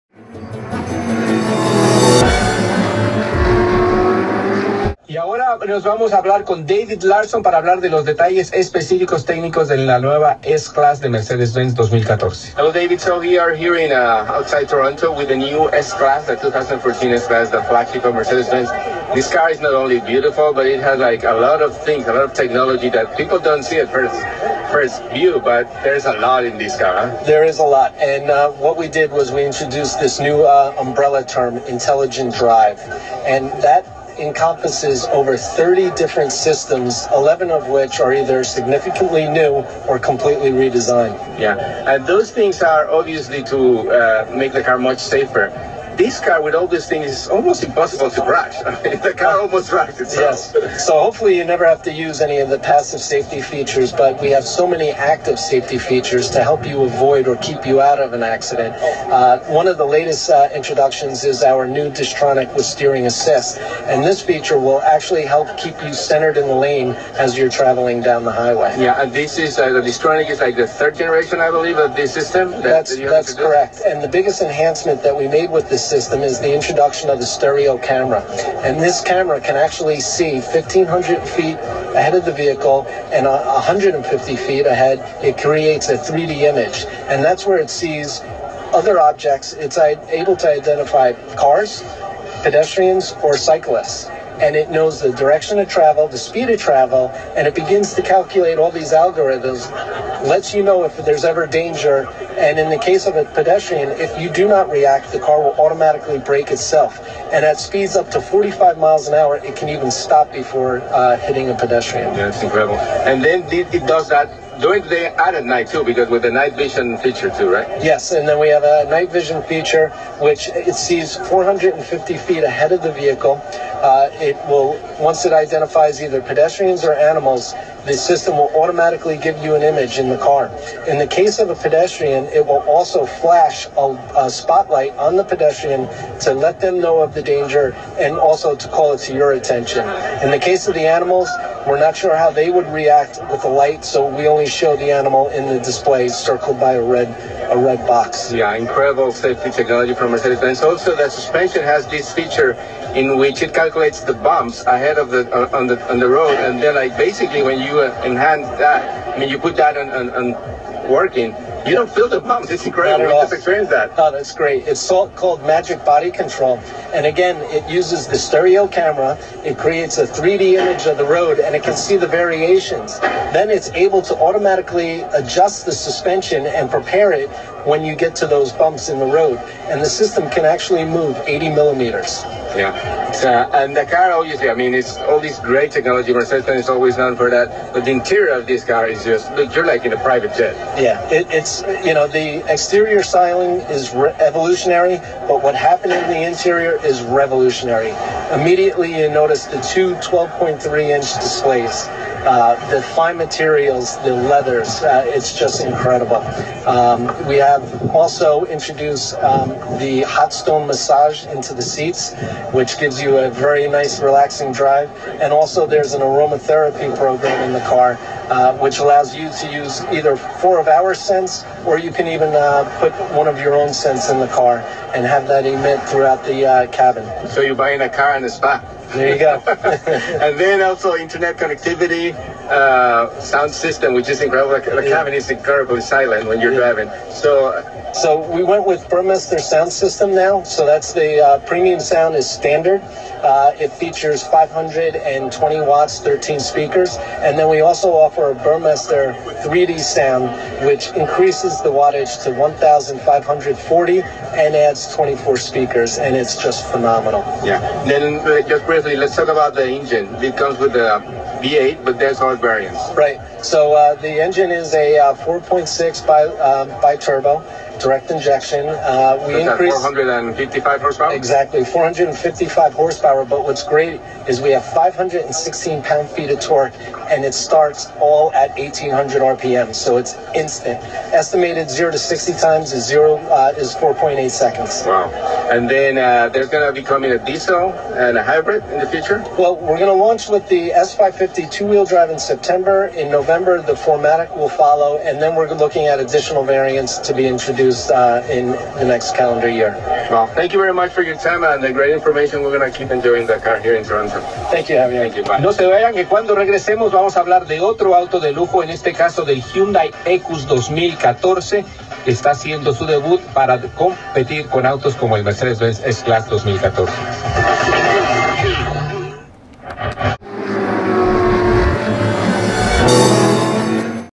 Autos 0-60: 2014 Mercedes-Benz S Class with Product Manager David Larsen
2014 Mercedes-Benz S Class with Product Manager David Larsen